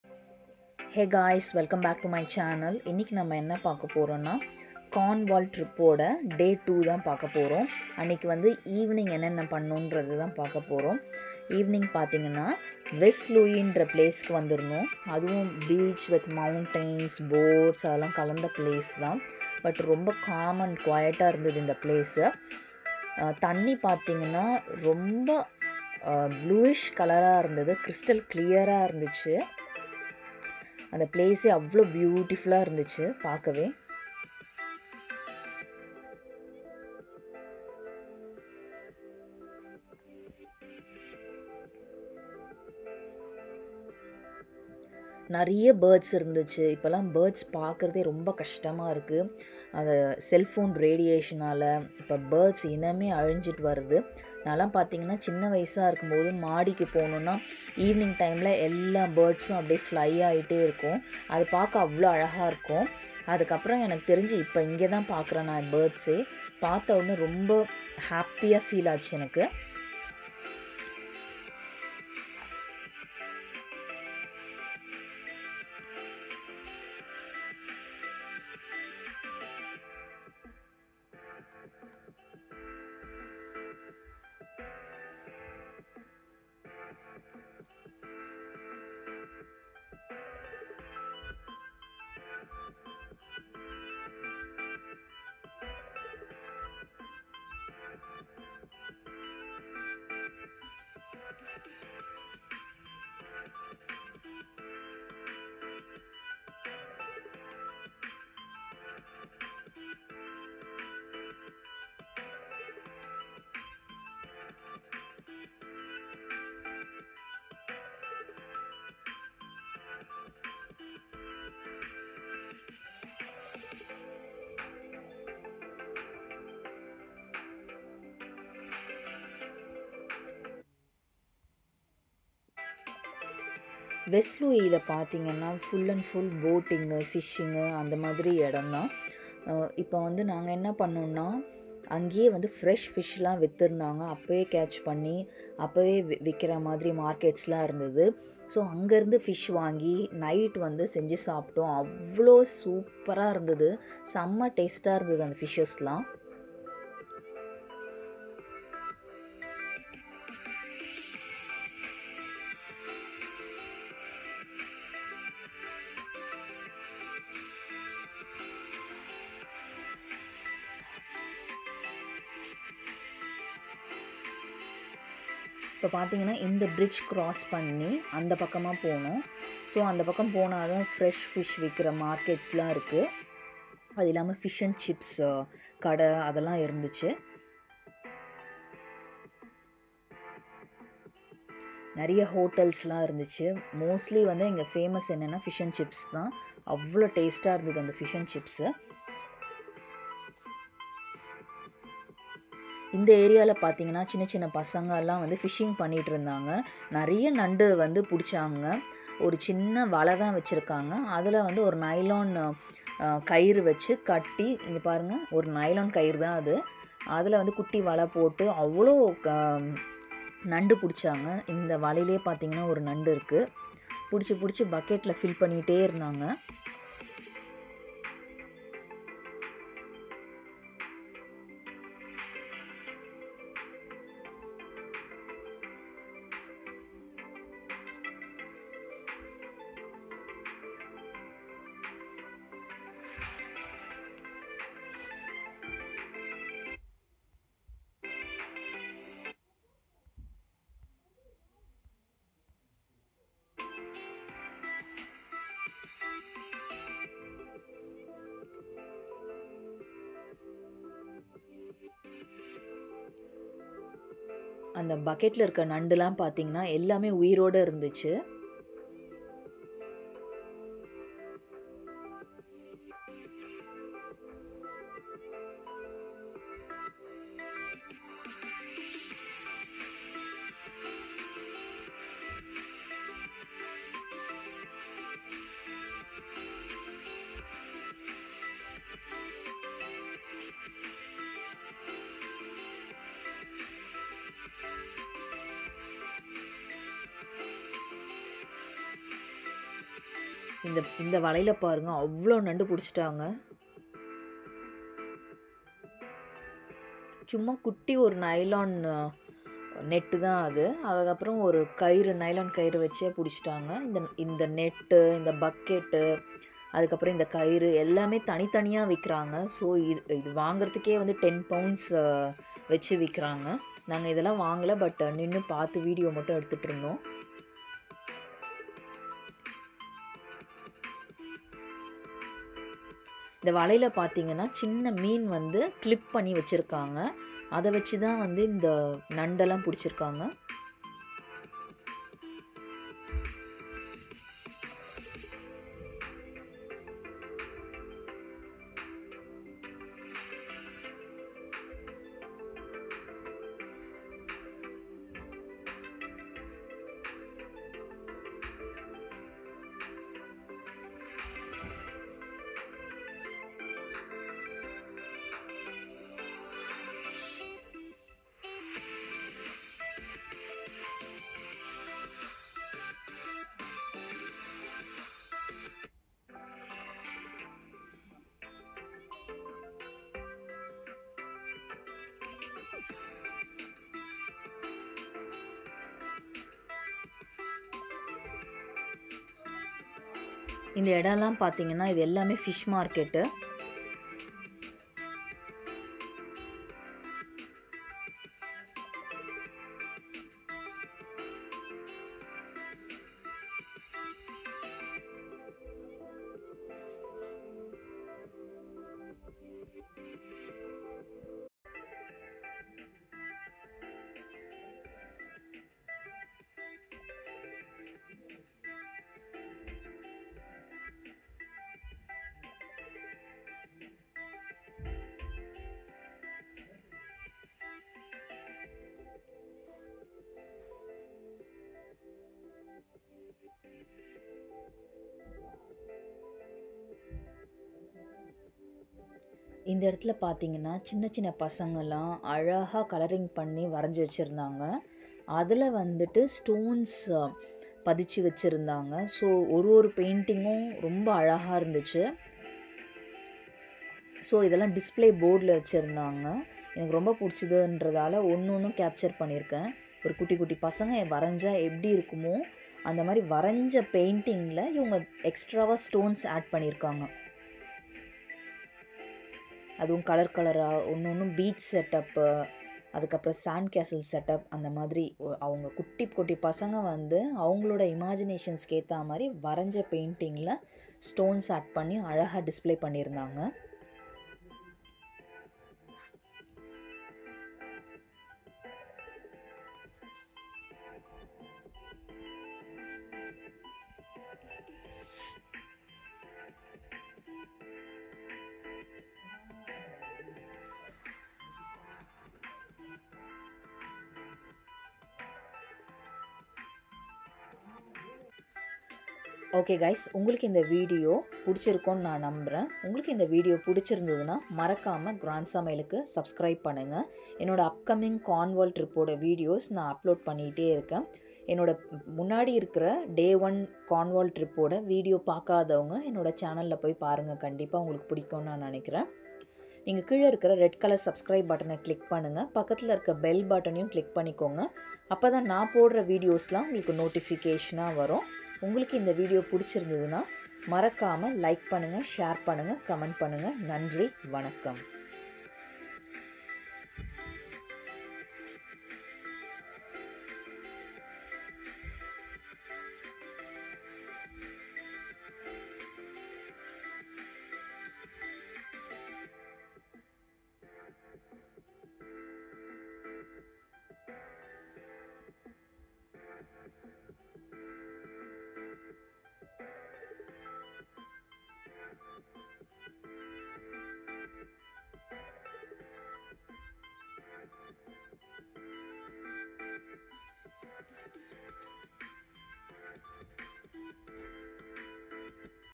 Day-2 Tamil Vlog - Trip to Cornwall WEST LOOE
Hey guys we went to West Looe to explore the beautiful fishing sea, Guys it was amazing and did see the kids are fishing with a small net.